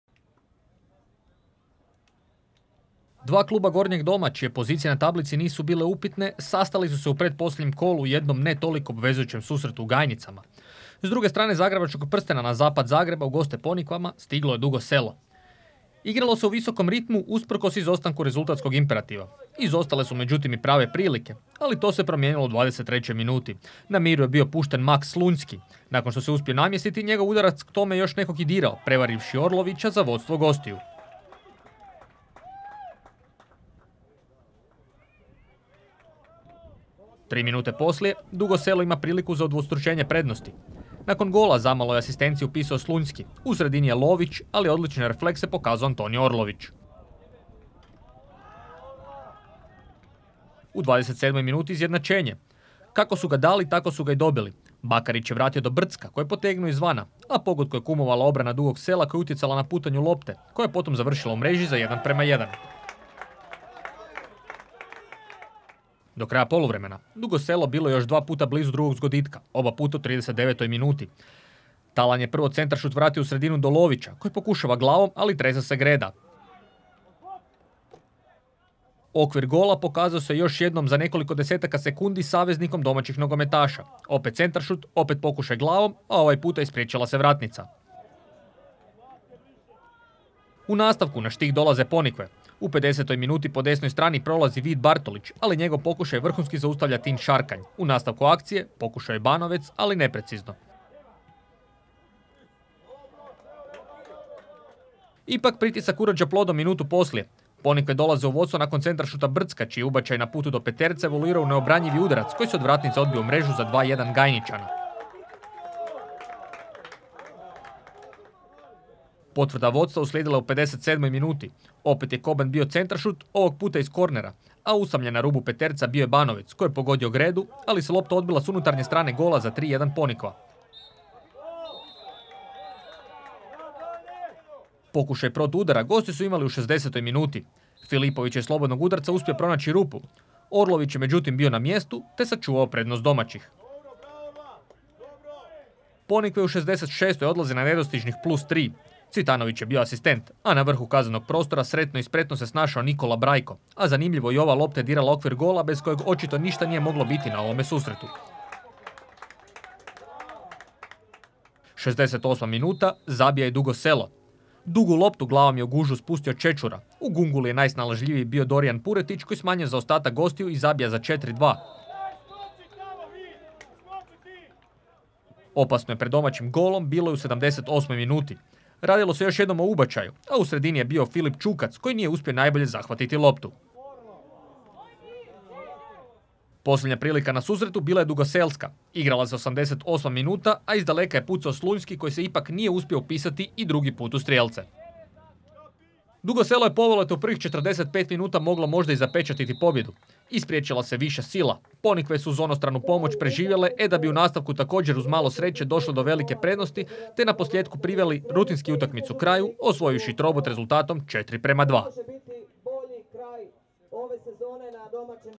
PONIKVE vs DUGO SELO 4:2 (33. kolo, Treća HNL - Središte 20/21)
Sažetak utakmice 33. kola Treće HNL - Središte između Ponikva i Dugog Sela (4:2).